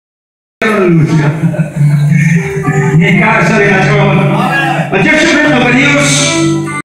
Nathan an Abigail's wedding june 18,2017
it took place at red bank village stann creek district (belize)